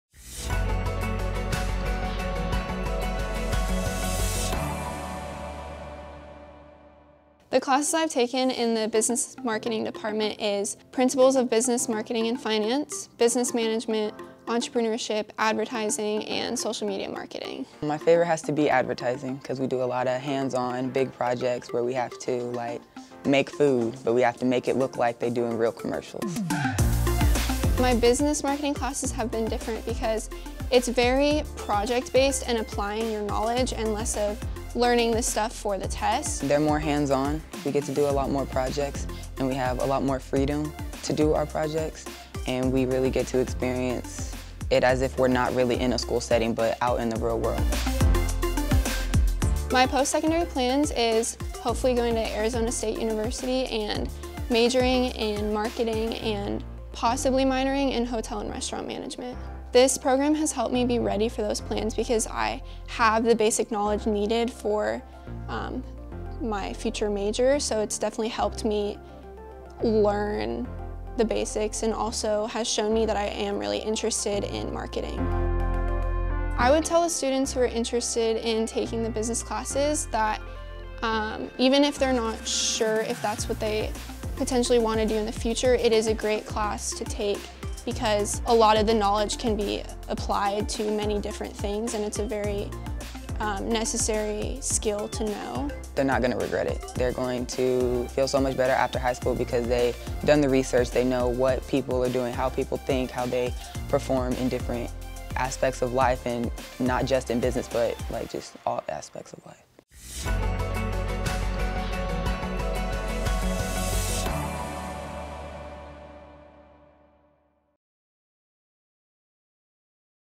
CTE in 1LISD: Business Marketing & Sales
Learn more about Career and Technical Education (CTE) in Leander ISD